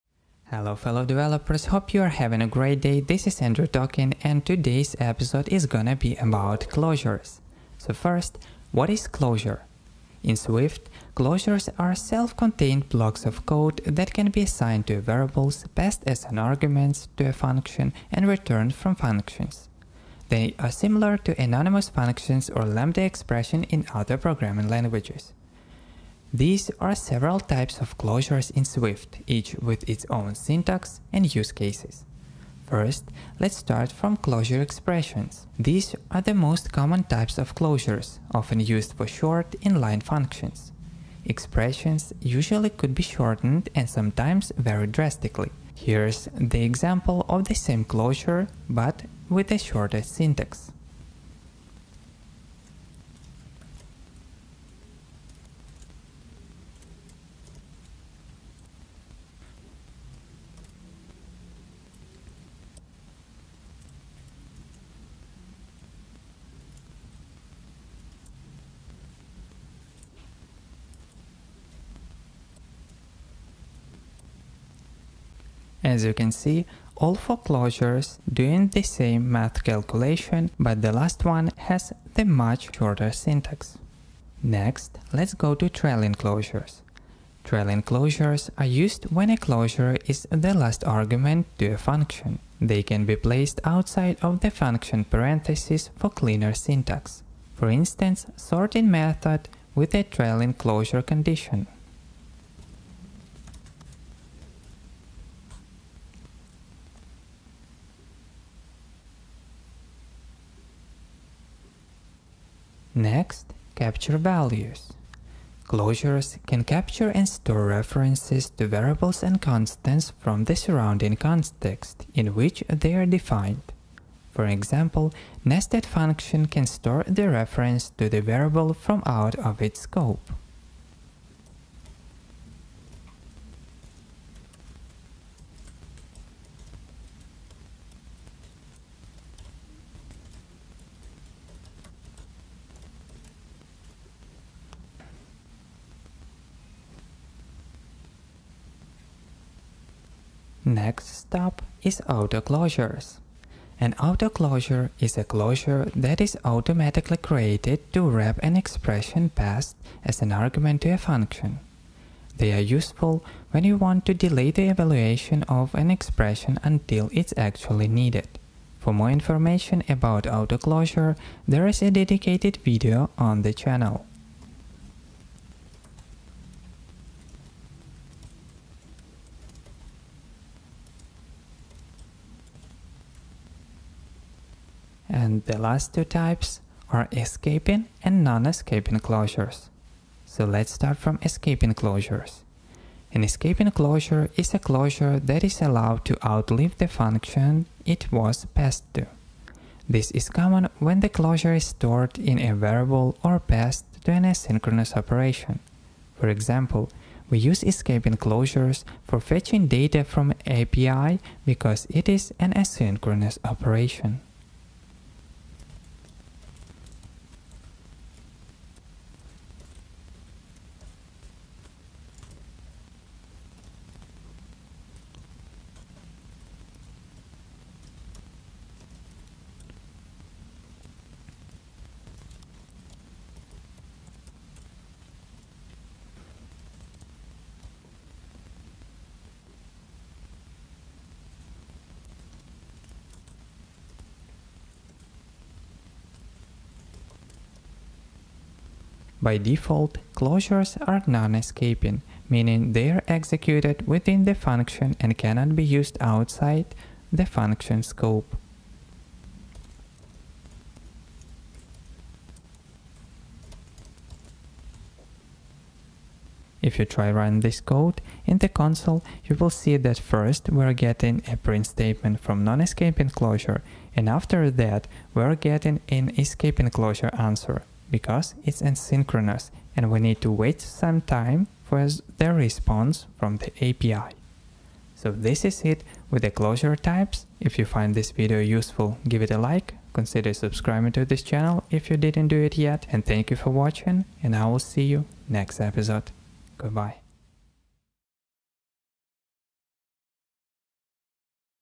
Types of Closures in Swift
In this video you will find out types of closures in Swift.

TIMECODES
00:00 - Intro
00:34 - Closure Expressions
01:24 - Trailing Closures
01:52 - Capture Values
02:30 - Autoclosures
03:10 - Escaping Closures
04:09 - Non-Escaping Closures
04:45 - Outro